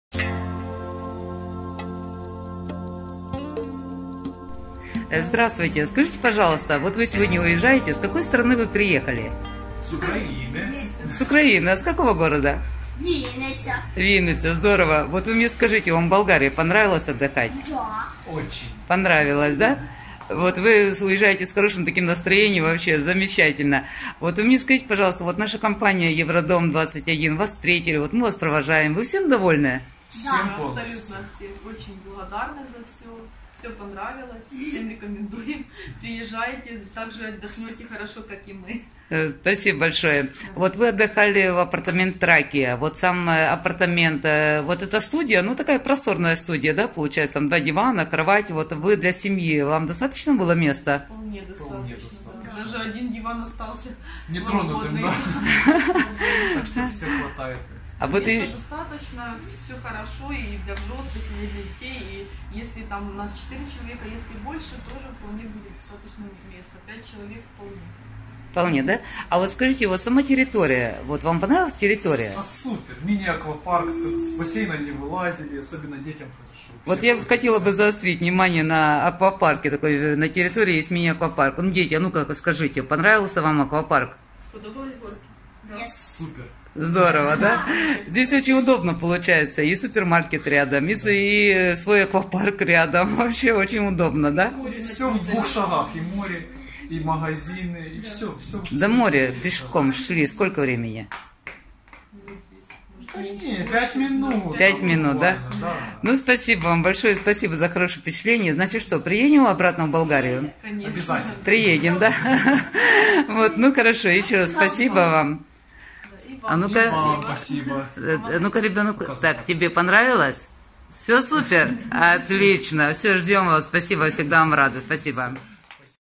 Отзыв туристов об отдыхе. Солнечный берег, комплекс Trakia. 10.08.2019
Отзыв туристов об отдыхе в Болгарии и проживании в комплексе Trakia, Солнечный берег. Компания ЕвроДом21 - аренда любых видов недвижимости в Болгарии.